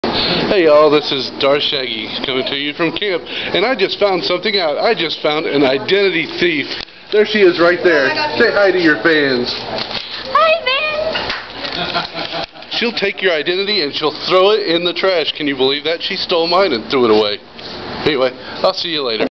We had a identity theaf at camp!! 7/27/2010
Watch out for this girl!! She will steal your identity, and toss it in the trash. That is what she did to me. She took one of my name tags, and threw it away!! SHAME!!!